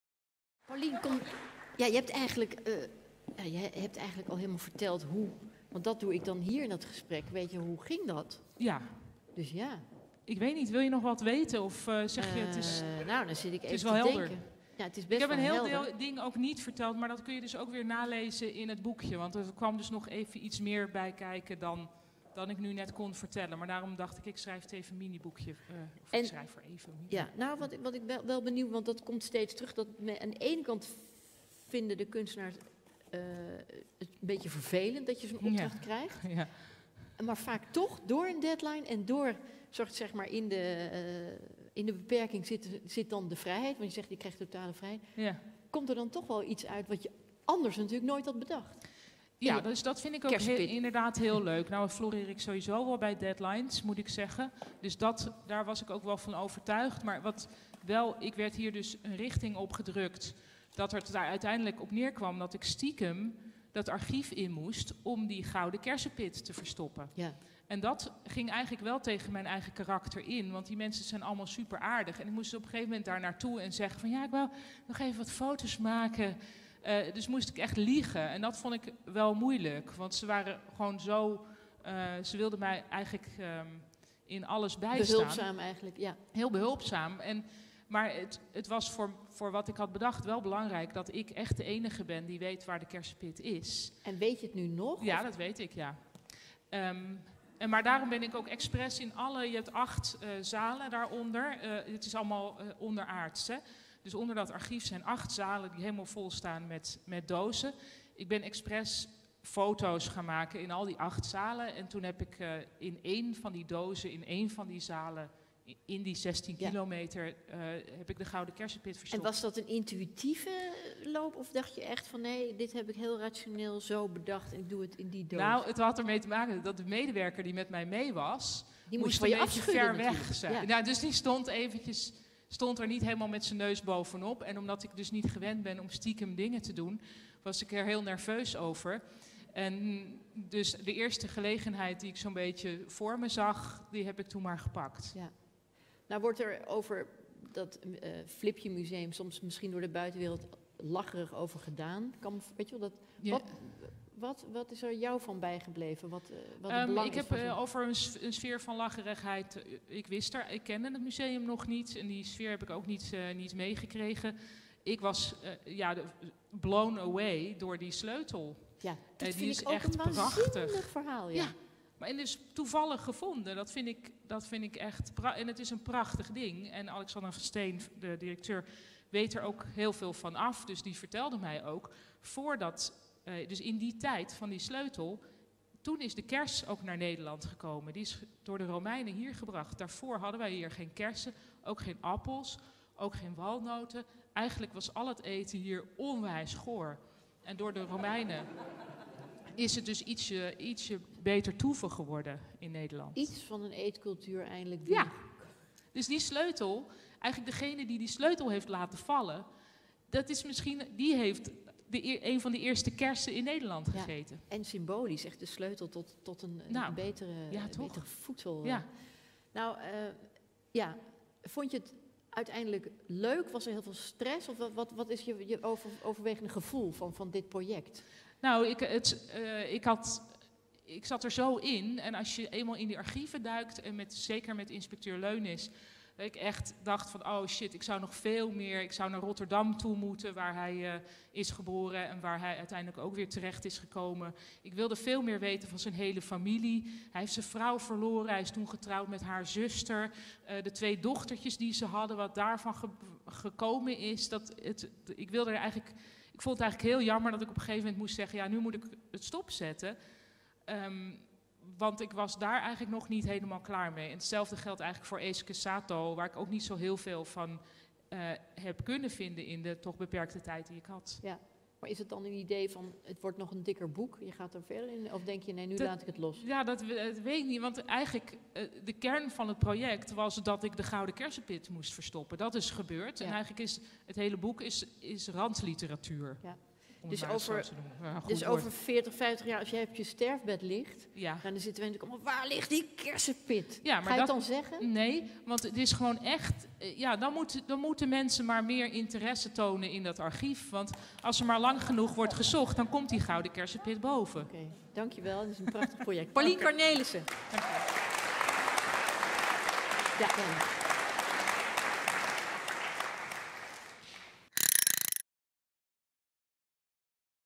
ERFGOEDFESTIVAL | Interview met Paulien Cornelisse
Andrea van Pol praat na met Paulien Cornelisse over het nieuwe werk dat zij maakte geïnspireerd door haar residentie voor het Erfgoedfestival 2020.